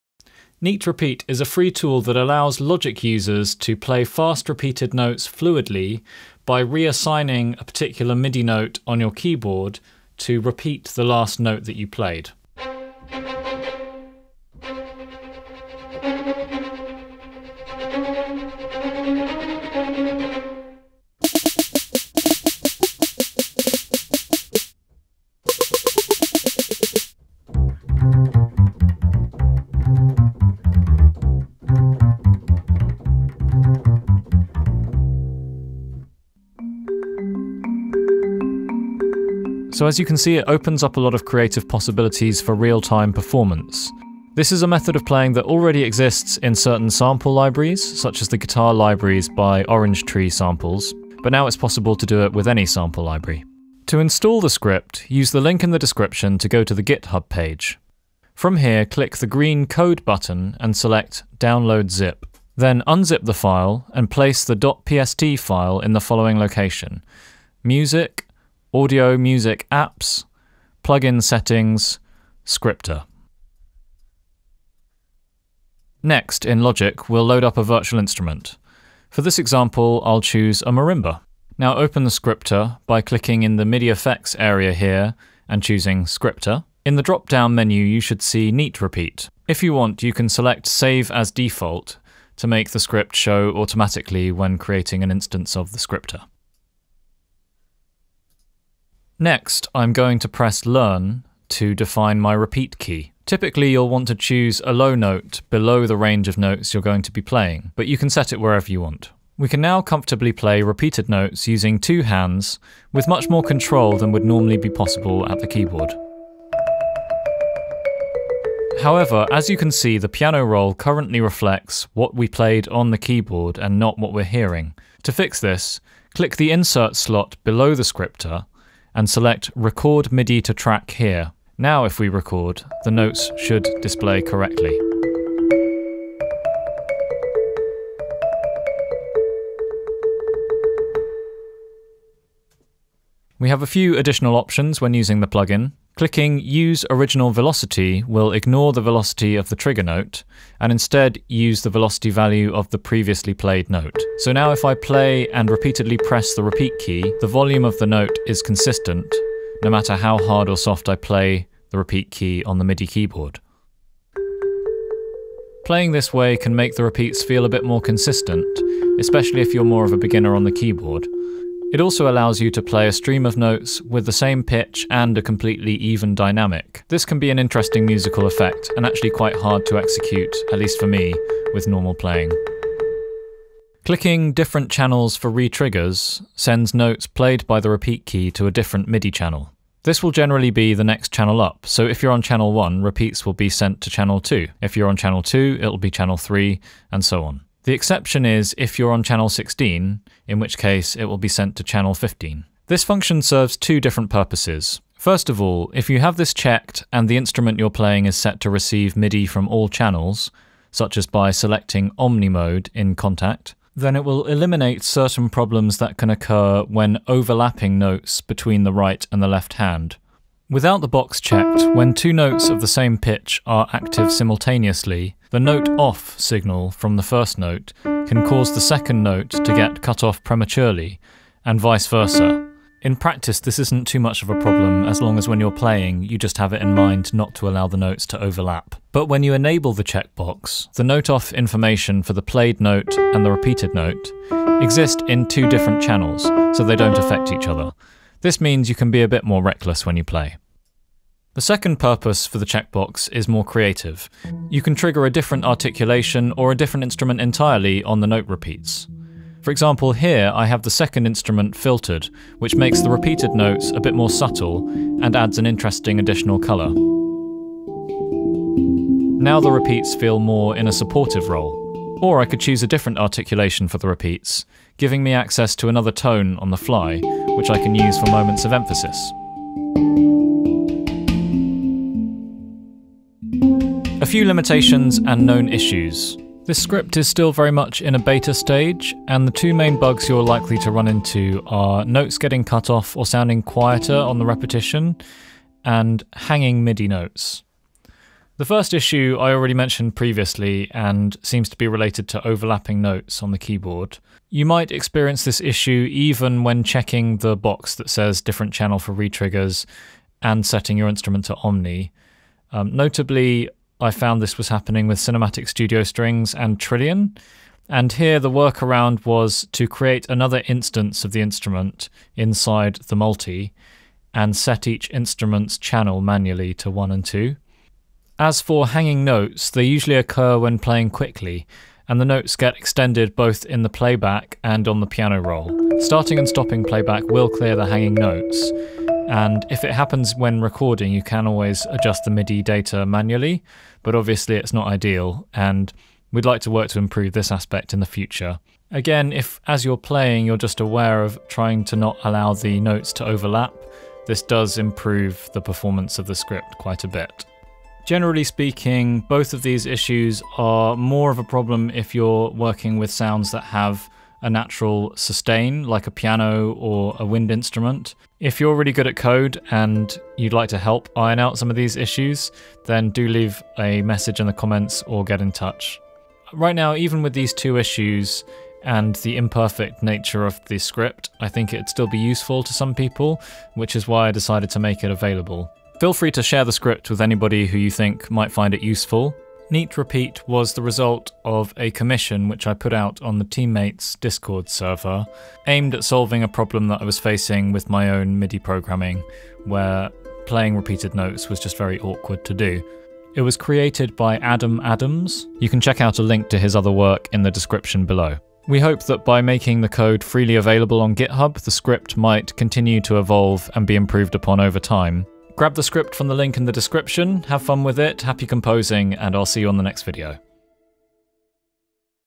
Play fast repeated notes with ease (NeatRepeat)
We created a script that works with any sample library within Logic, allowing you to play fast repeated notes with a level of comfort and control that isn't normally possible on the keyboard.

Click Code: 'Download Zip'

Copy the .pst file to Music / Audio Music Apps / Plug-In Settings / Scripter

Open the Scripter plugin in logic and select NeatRepeat from the drop down menu.

Created by Adam Adams

Timecodes